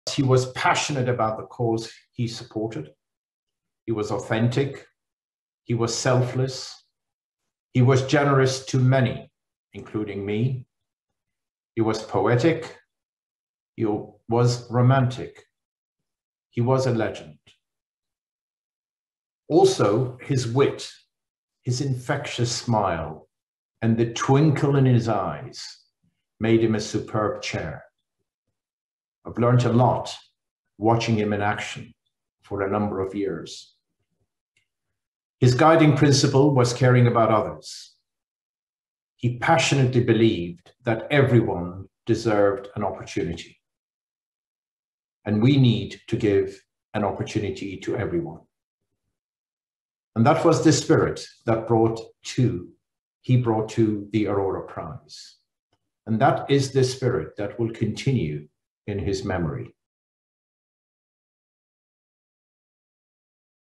Aurora Dialogues | Լորդ Արա Դարզի
Լորդ Արա Դարզի՝ «Ավրորա» մրցանակի Ընտրող հանձնաժողովի նախագահ. «Վարդան Գրեգորյանը պոետիկ էր, ռոմանտիկ, նա լեգենդ էր: Տարիներ շարունակ նրան հետեւելով՝ ես շատ բան եմ սովորել»